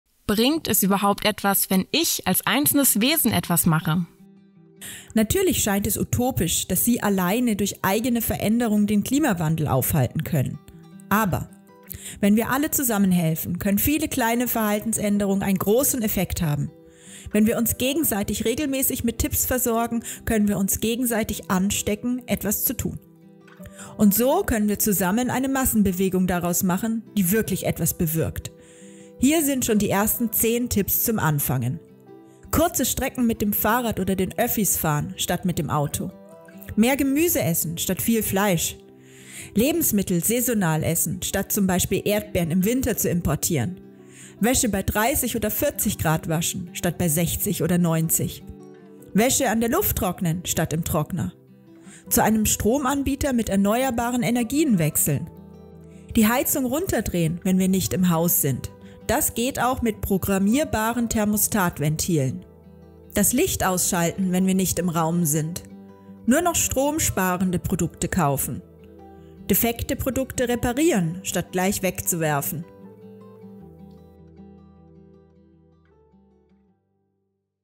ClimatePD Die Auswirkung des Einzelnen auf den Klimawandel
Was kann ich als einzelne Person gegen den Klimawandel tun?